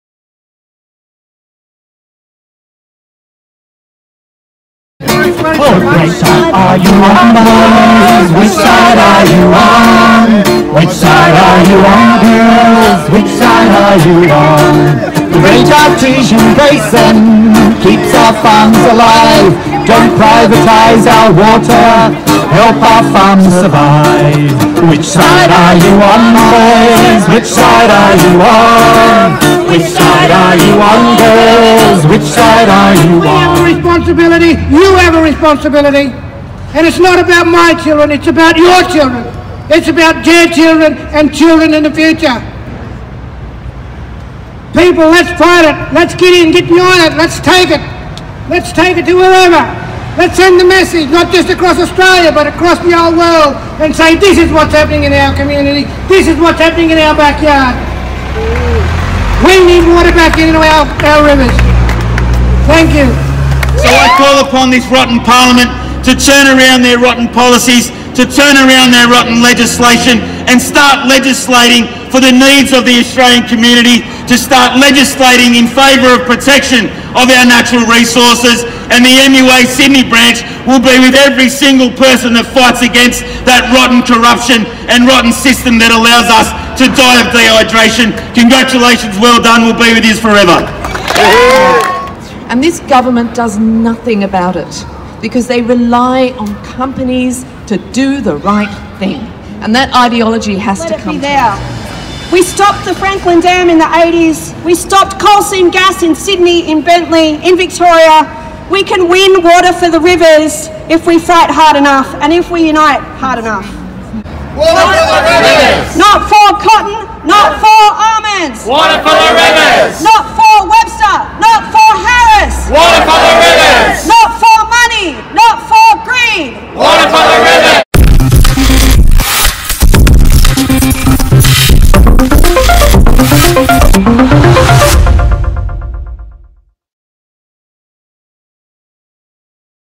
Water for the rivers!
The Murray-Darling Basin river system is in deep crisis. The rivers are dying, communities (many mainly Indigenous) are running out of drinkable water and there are mass fish kills following blue-green algae. Largescale corporate farming and profiteering from privatised water rights has added to the problem of climate change. This protest called on the NSW government  to: leave water in the rivers; stop irrigation or mining pumping during low to medium flows; strip water licenses from water thieves; ban floodplain harvesting; and assist farmers to grow crops better suited to arid climates. Video by Peter Boyle for Green Left Weekly.